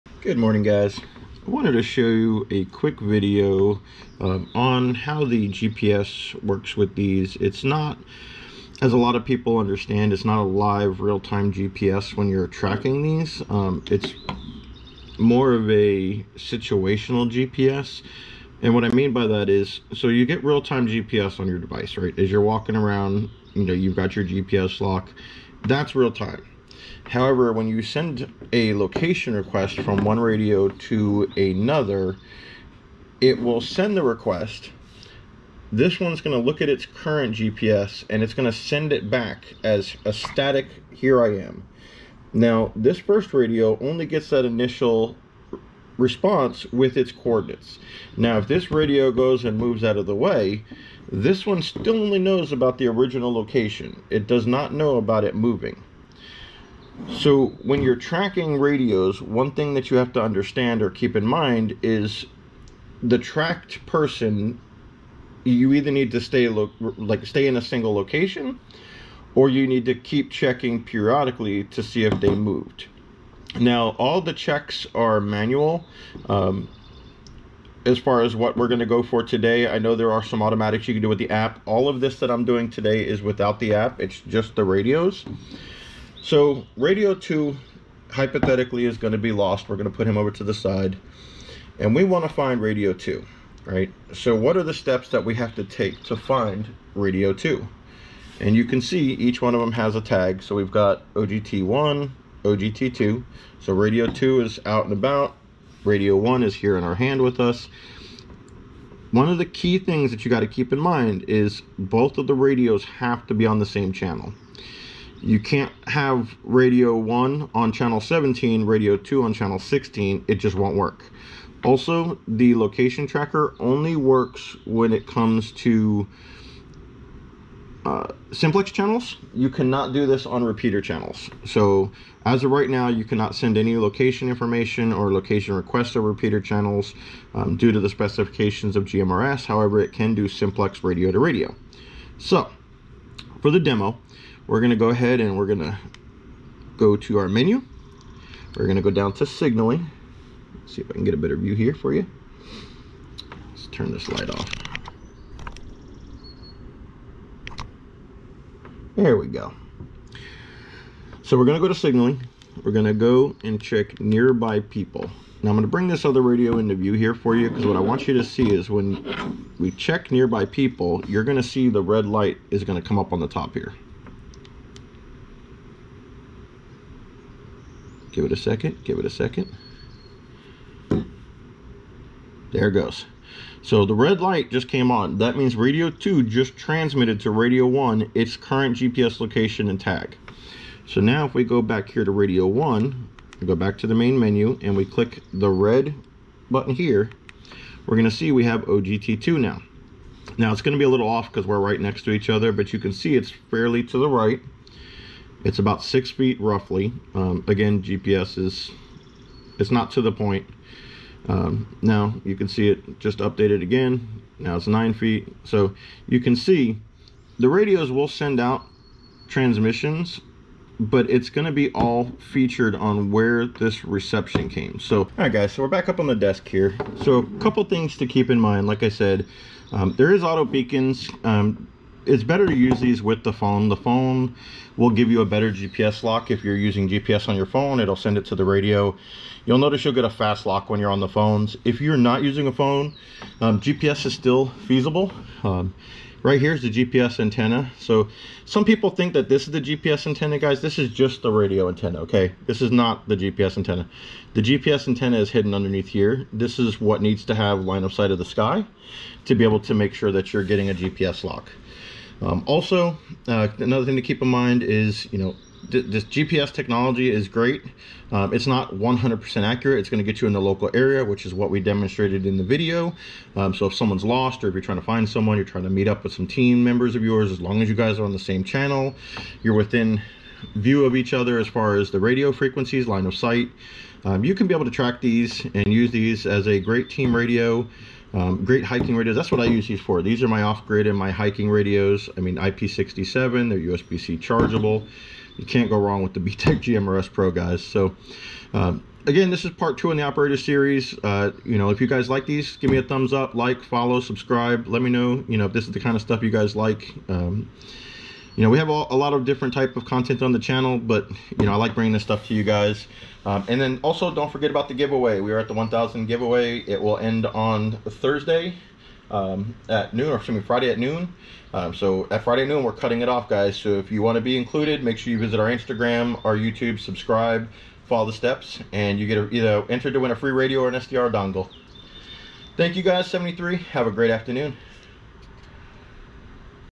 Operator Series: GMRS-PRO Locate partner without phones | Offgrid Technology
Today we continue with the Operator Series: GMRS-PRO. In today's video, we will be using the 2x BTECH GMRS-Pro to demonstrate how to ping and navigate (small demo) to a partner using a GMRS-Pro. A few key notes which we discuss in the video is that both radios need to be on the same Simplex Channel (You cannot use Repeater Pairs) to send and receive the data bursts.

If you do not yet have a GMRS-Pro you can get one from BTECH

If you like our videos and want to help us purchase new products to bring to you guys please consider buying me a beer or coffee :)